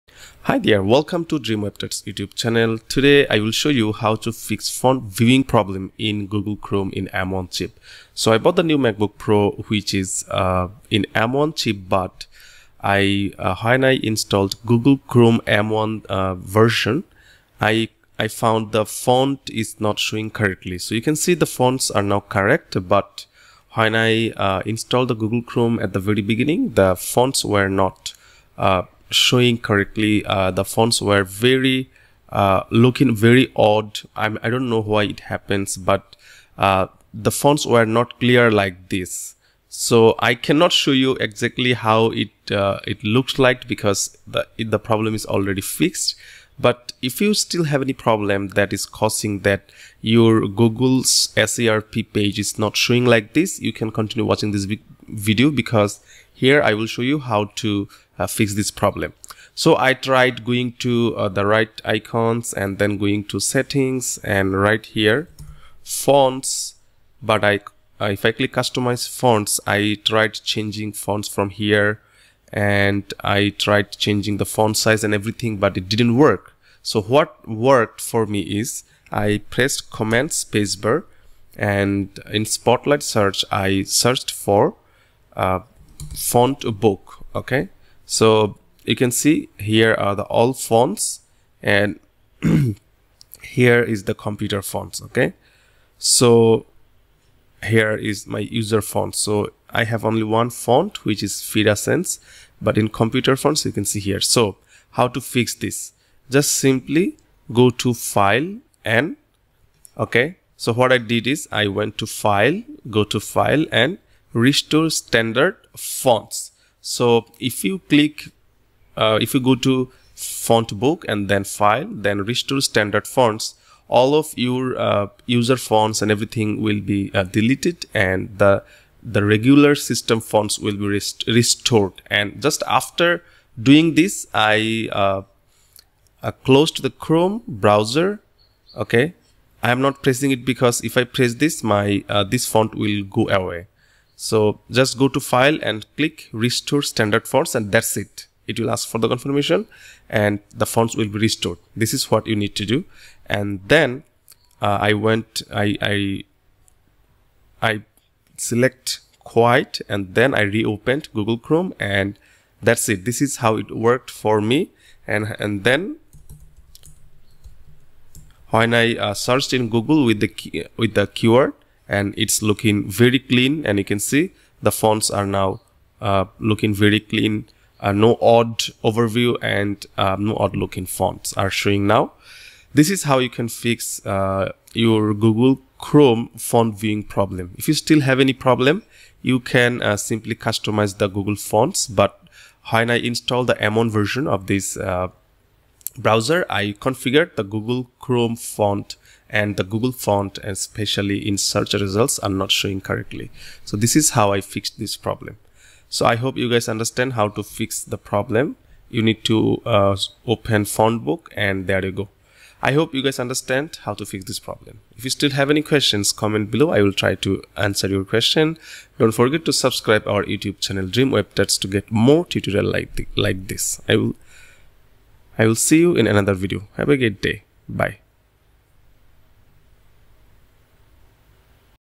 How to Fix Google Chrome Font Problem in Macbook Pro M1
In macbook Pro m1, I found the fonts are strange. When I search something, fonts are not showing correctly. It looks very odd. I tried changing the settings, fonts size etc in Google Chrome but didn't work. I was very hopeless, I searched in Google for the solution but it didn't work as well.

So I thought myself that I need to restore the system fonts because it is causing conflicts in arial fonts. So I went to Fontbook and selected Restore System Fonts. Then it worked immediately. How cool is that?